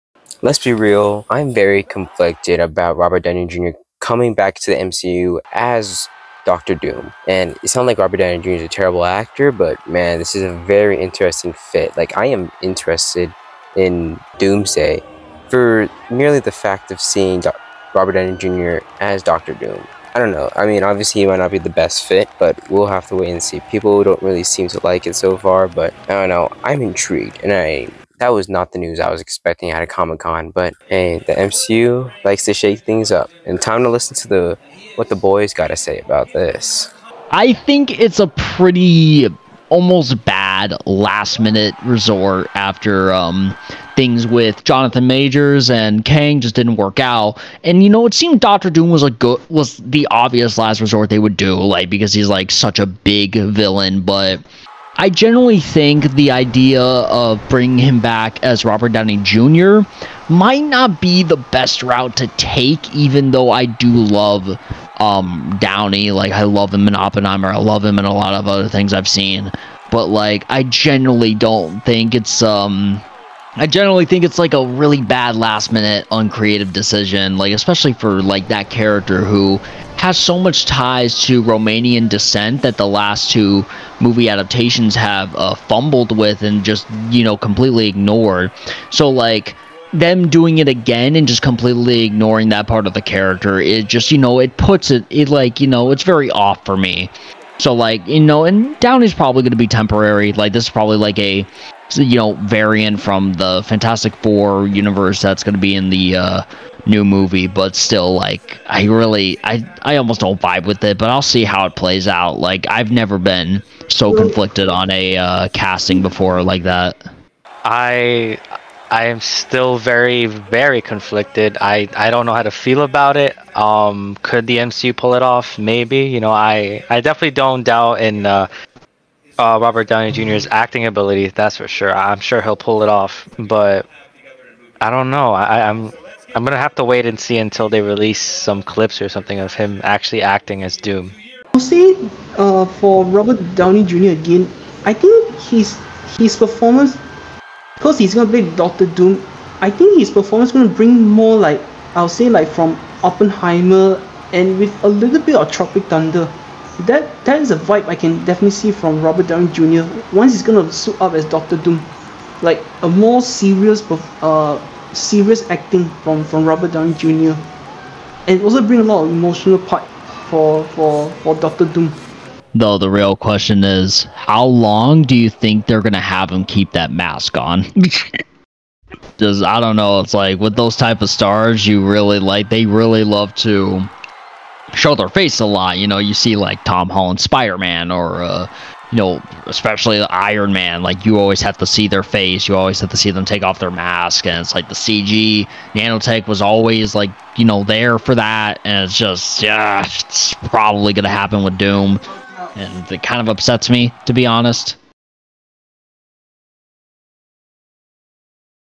ROBERT DOWNEY JR. WILL KILL IT AS DR. DOOM & WE'RE STILL EXTREMELY CONCERNED | REEL ONES REACT PT. 1
SHOUTOUT TO THE REEL ONES IN THE DISCORD COMMUNITY TO CHIMING IN!
@CineflickProductions @randomp3rson999 @eugene tan
hope to do it again my friends!

this is probably the biggest comic con announcement in a long time
and the most conflicting comic book casting ever!
nonetheless, robert downey jr. got that AURA!